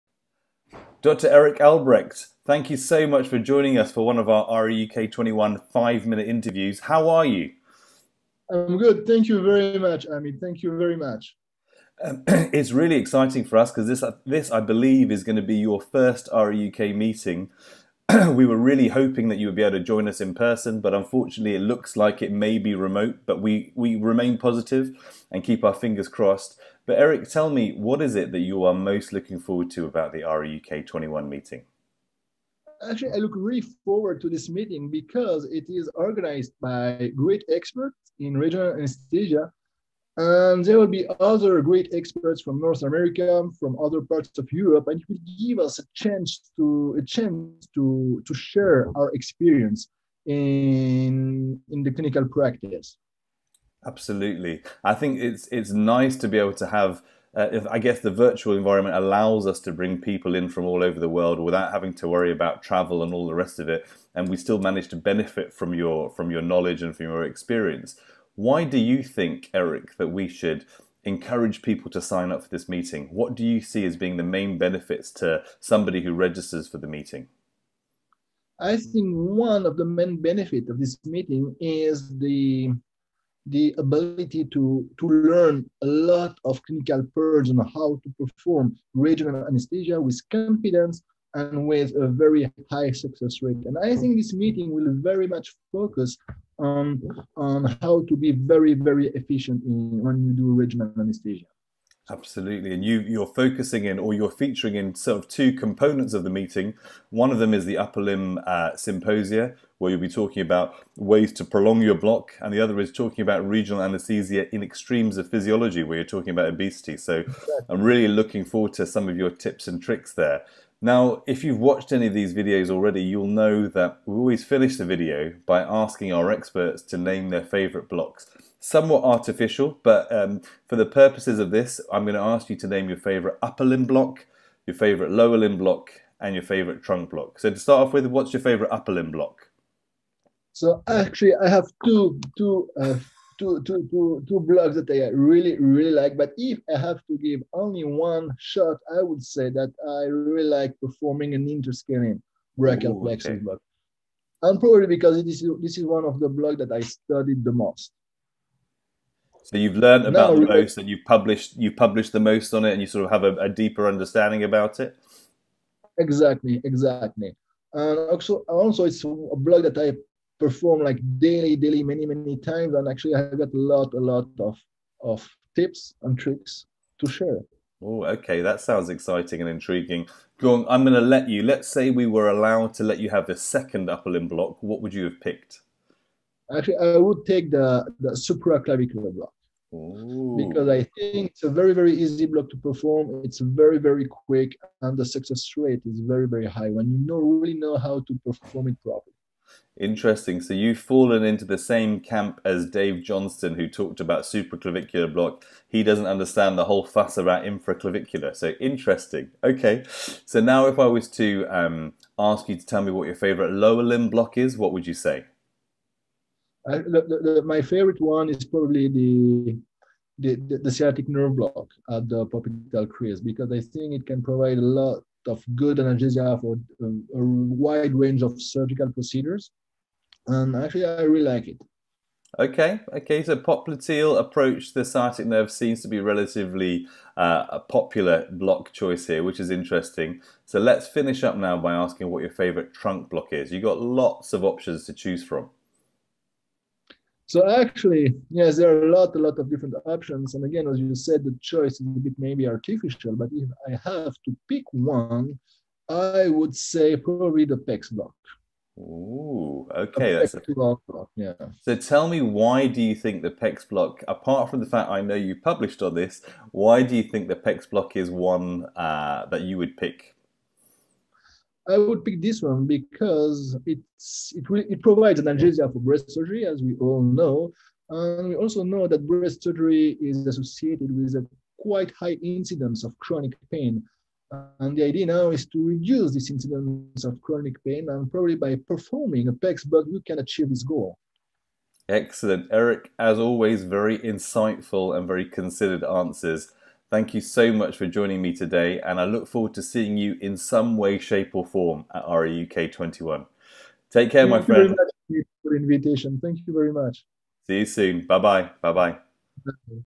Dr Eric Albrecht RAUK21 Five minute interview
In this brief interview, Dr Amit Pawa, RA-UK President catches up with Dr Eric Albrecht to find out why he is excited about RAUK21 and why he thinks people should register! Can you guess what his favourite blocks are?

Uploaded on 25 Mar 2021